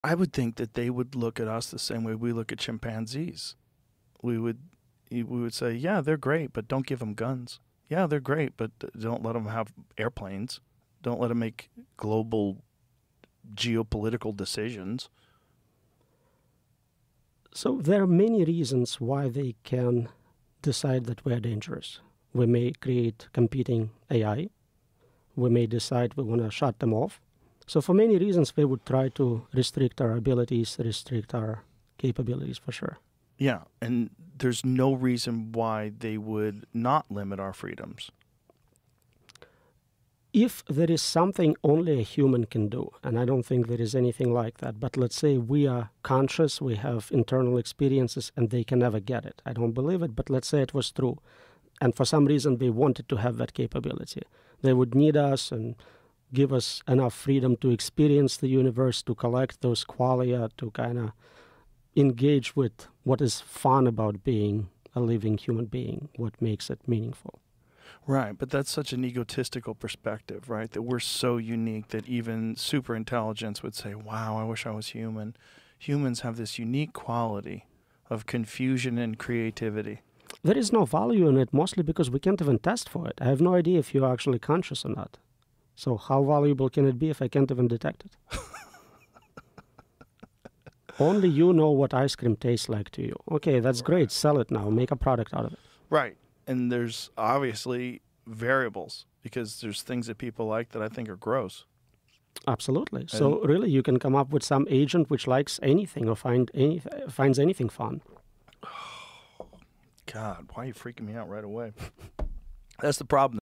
Why Superintelligence Would Disempower Us
Why would superintelligence take measures to restrict our abilities?

Roman Yampolskiy: "There are many reasons why they can decide that we're dangerous. We may create competing AI, we may decide we want to shut them off..."